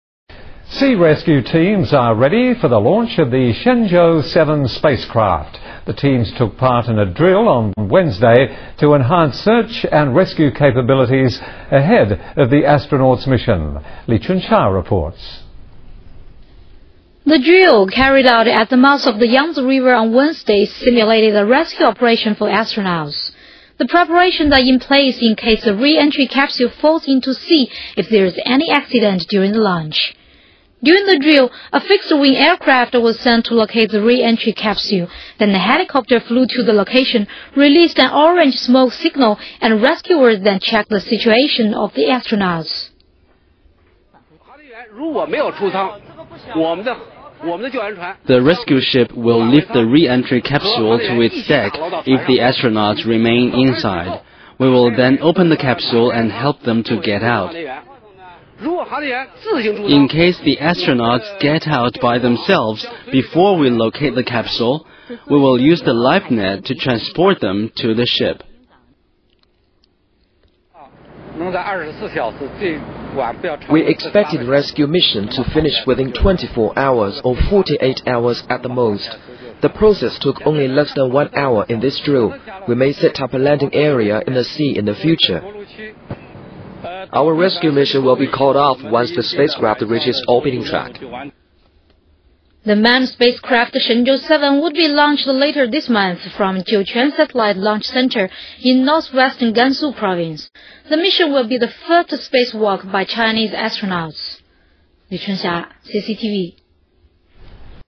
Sea rescue ready for Shenzhou VII
Sea rescue team ready for Shenzhou 7 mission
WATCH VIDEO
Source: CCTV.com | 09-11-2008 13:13
Special Report:   Shenzhou 7

Sea rescue teams are ready for the launch of the Shenzhou-7 spacecraft. The teams took part in a drill on Wednesday, to enhance search and rescue capabilities ahead of the astronauts' mission.
The drill, carried out at the mouth of the Yangtze River on Wednesday afternoon, simulated a rescue operation for astronauts. The preparations are in place, in case the re-entry capsule falls into sea if any there is an accident during the launch.

During the drill, a fixed-wing aircraft was sent to locate the re-entry capsule. Then a helicopter flew to the location, released an orange smoke signal, and rescuers then checked the situation of the "astronauts".

Zhu Baozhu, Vice Director of Sea Rescue Program for Shenzhou 7 said "The rescue ship will lift the re-entry capsule to its deck if the astronauts remain inside. We will then open the capsule and help them to get out. In case the astronauts get out by themselves before we locate the capsule, we will use the life net to transport them to the ship."
Zhang Jianqi, Vice Director of Shenzhou 7 Space Program said "We expected the rescue mission to finish within 24 hours, or 48 hours at the most. The process took only less than one hour in this drill. We may set up a landing area in the sea in the future."

Xu Zuyuan, Vice Minister of Transport Ministry said "Our rescue mission will be called off once the spacecraft reaches its orbiting track."

The manned spacecraft Shenzhou-7 would be launched later this month from the Jiuquan Satellite Launch Center in northwestern Gansu Province. The mission will be the first space walk by Chinese astronauts.